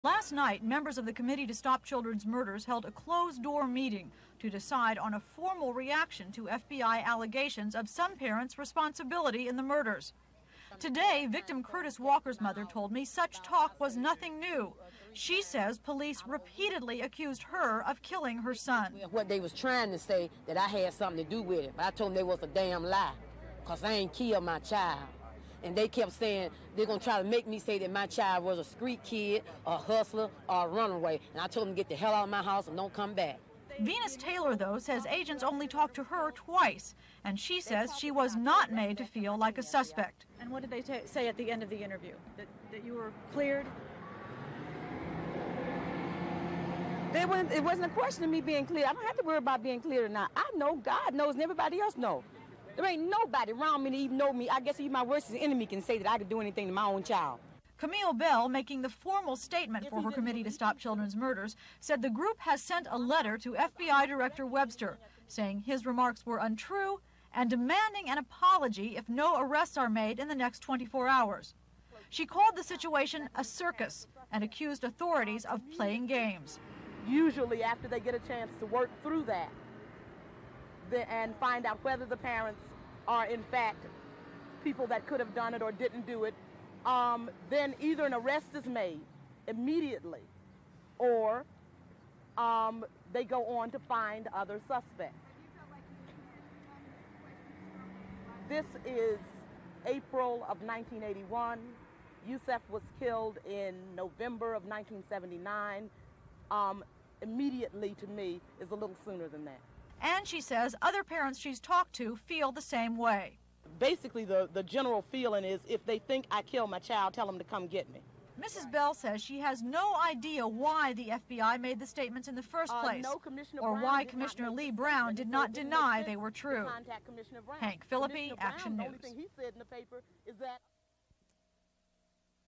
Atlanta Child Murder Victims' Mothers React to FBI Assertion That Some Parents Involved (4/19/81)
Voiceover: A report on a meeting of the Committee to Stop Children's Murders in response to the FBI's assertion that some parents might be partially responsible for the deaths of their children and the Committee’s official statement; interviews with murdered child Curtis Walker’s mother and Venus Taylor. Footage: Women walking into the meeting; Camille Bell, mother of murdered child Yusuf Bell, in a press conference.
Atlanta Missing and Murdered Children Story
Reporter: Phillippi, Hank